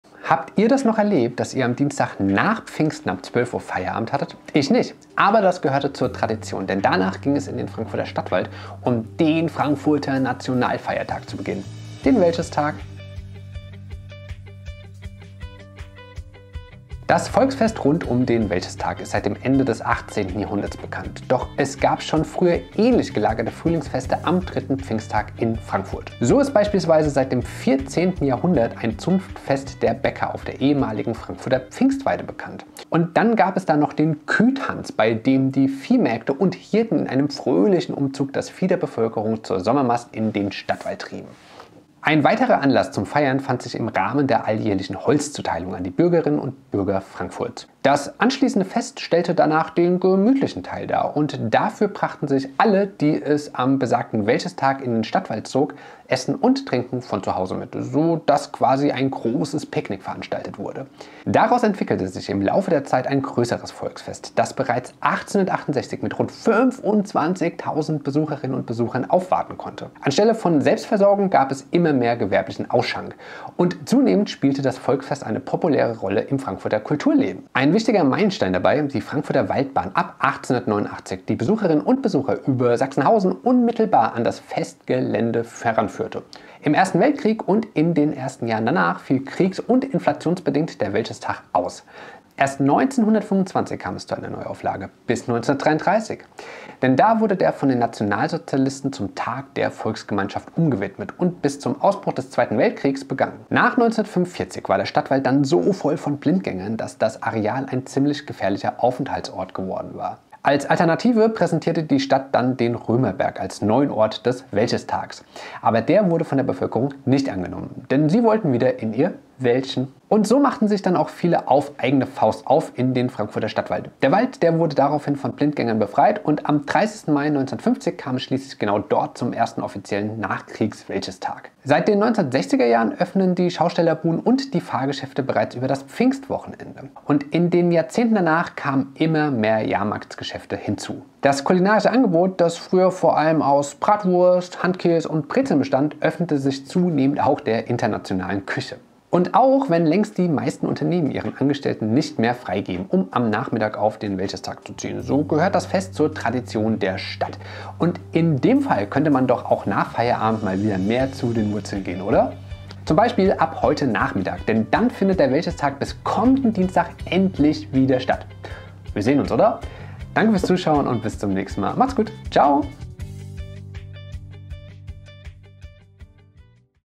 Der Wäldchestag und seine Geschichte
Habt ihr das noch erlebt, dass ihr am Dienstag nach Pfingsten ab 12:00 Uhr Feierabend hattet? Ich nicht. Aber das gehörte zur Tradition – denn danach ging es in den Frankfurter Stadtwald um den Frankfurter Nationalfeiertag zu begehen: Den Wäldchestag.